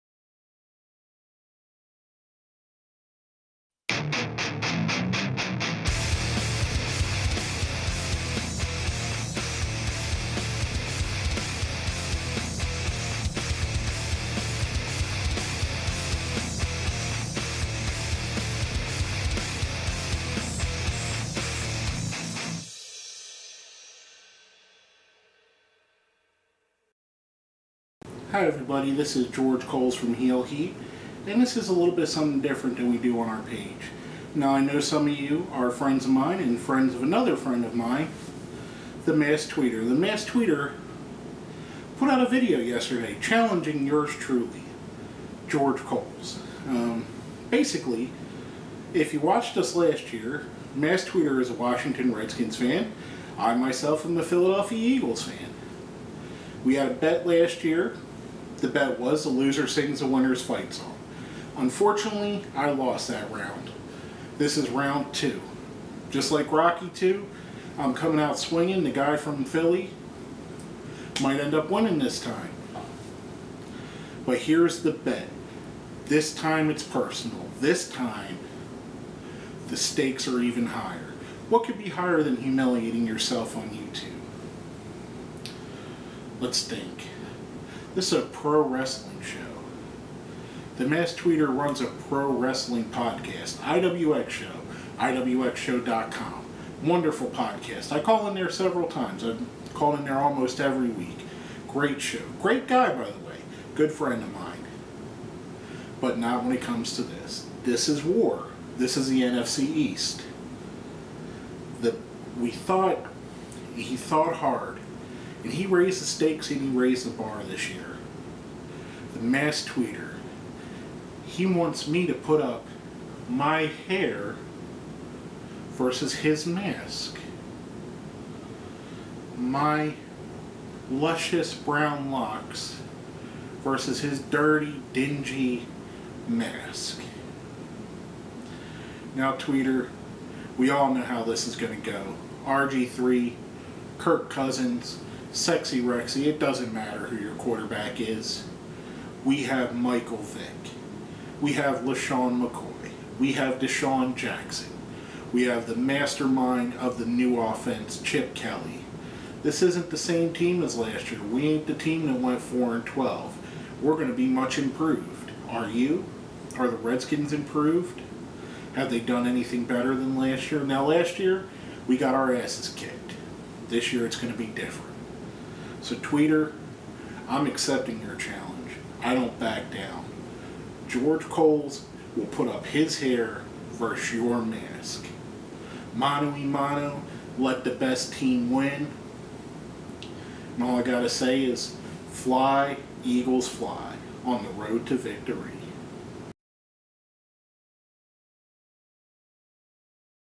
Eagles Fan Vs Redskins Fan BET 2013 Hair vs Mask
The Masked Tweeter threw out a challenge to George and this is his response